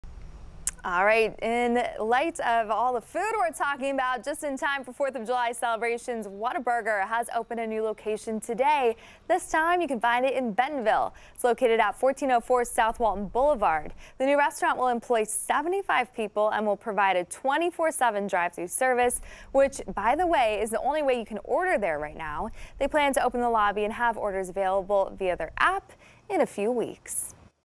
Bentonville Whataburger opens for business
Just in time for Fourth of July celebrations, Whataburger has opened a new location on South Walton Boulevard in Bentonville.